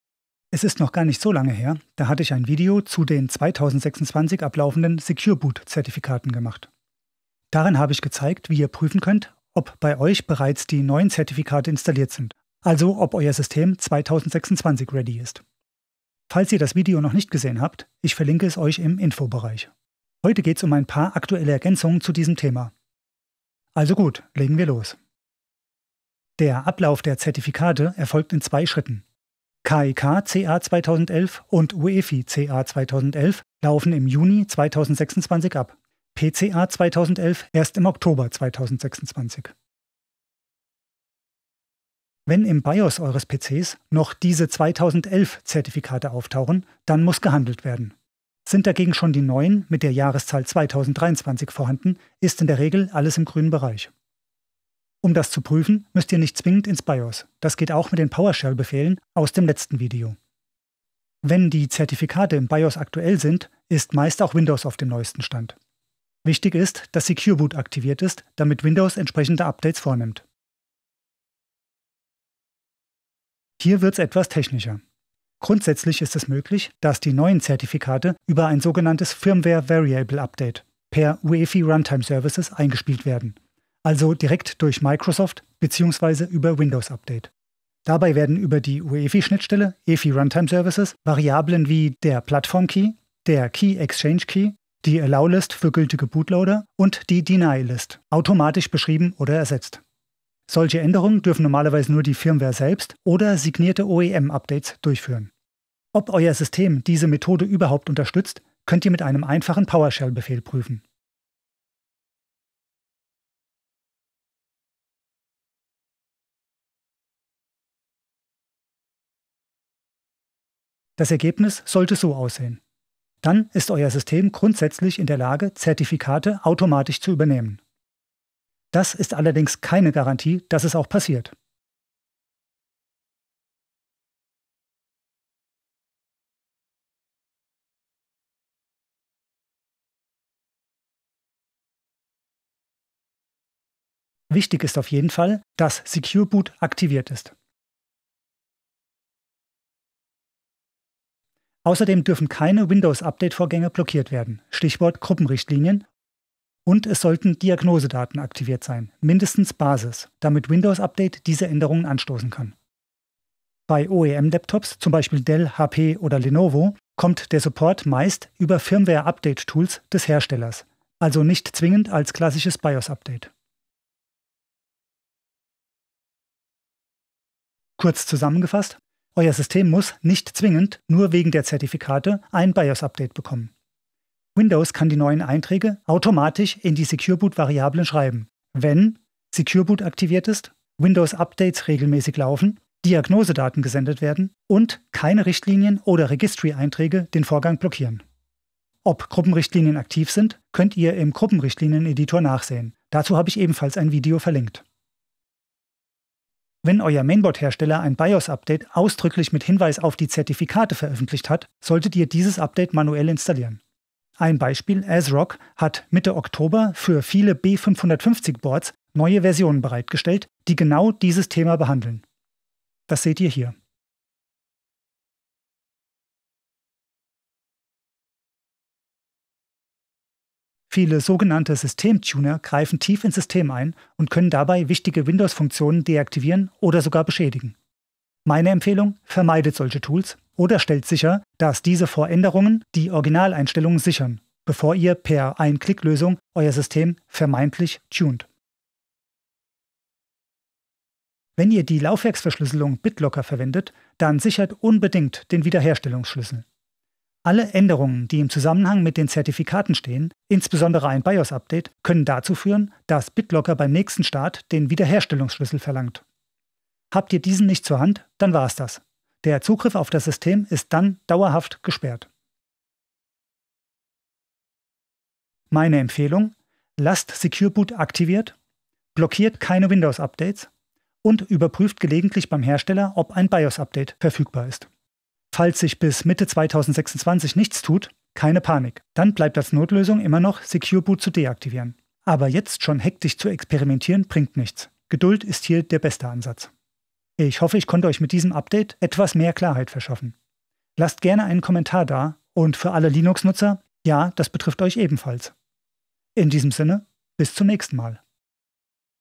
Microsoft CA 2023 Zertifikate – Update & neue Infos zu Secure Boot und 2026!
Im Juni und Oktober 2026 laufen die aktuellen Secure-Boot-Zertifikate von 2011 ab. Viele neuere PCs erhalten die neuen Zertifikate direkt über Windows Update. Bei weniger gepflegten Systemen kann es jedoch passieren, dass der Anwender selbst Hand anlegen muss. In diesem Video erfahrt ihr, wie ihr den Zustand eures Systems überprüft und welche Optionen ihr habt.

👉 PowerShell-Befehl (aus diesem Video):
Get-SecureBootUEFI -Name PK | Format-List

👉  PowerShell-Befehle (aus dem letzten Video):
Confirm-SecureBootUEFI
[System.Text.Encoding]::ASCII.GetString((Get-SecureBootUEFI db).bytes) -match 'Windows UEFI CA 2023'

▬ Inhalt ▬▬▬
00:00 Einleitung
01:13 Firmware Update notwendig?
02:21 Wichtig für das Update
03:26 BIOS-Update verfügbar
03:49 System-Tuner Software
04:09 BitLocker-Hinweis

🛒 Einkaufen bei Amazon
*Wenn ihr diesen Link verwendet, bekomme ich eine Provision auf eure Bestellung.
Für euch ändert sich nichts! Vielen Dank, falls ihr den Link benutzt!